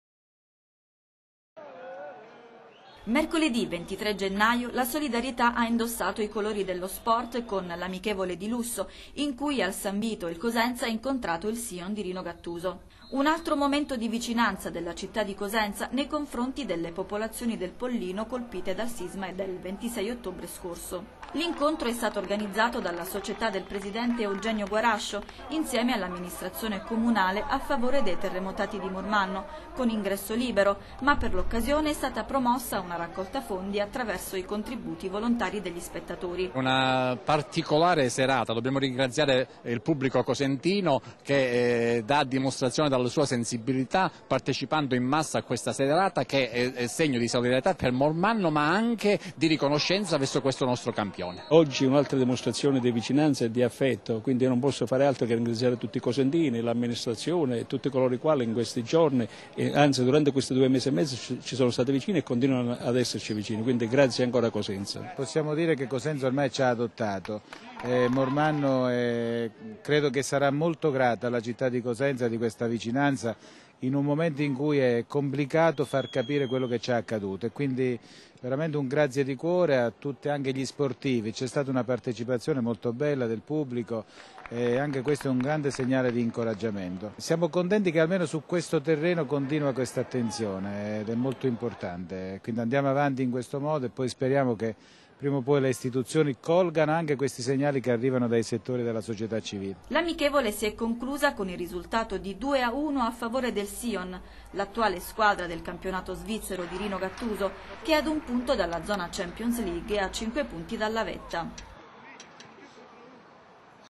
Cosenza: la partita della solidarietà con Gattuso
Mercoledì 23 gennaio la solidarietà ha indossato i colori dello sport, con l'amichevole di lusso in cui al San Vito il Cosenza ha incontrato il Sion di Rino Gattuso. Un altro momento di vicinanza della città di Cosenza nei confronti delle popolazioni del Pollino, colpite dal sisma del 26 ottobre scorso.
L'incontro è stato organizzato dalla società del Presidente Eugenio Guarascio, insieme all'amministrazione comunale, a favore dei terremotati di Mormanno, con ingresso libero. Ma per l'occasione è stata promossa una raccolta fondi, attraverso i contributi volontari degli spettatori. L'amichevole si è conclusa con il risultato di 2 a 1 a favore del Sion, l'attuale squadra del campionato svizzero di Rino Gattuso che è ad un punto dalla zona Champions League e a cinque punti dalla vetta.